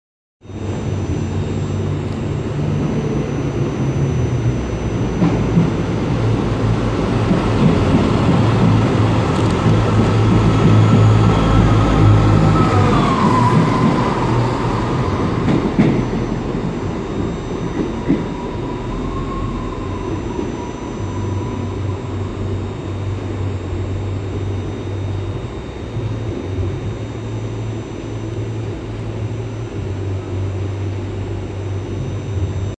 170393 Departing Waverley Ecs on an Edinburgh Waverley to Haymarket Tmd Working
170393 Departing Waverley Ecs on an Edinburgh Waverley to Haymarket Tmd Working.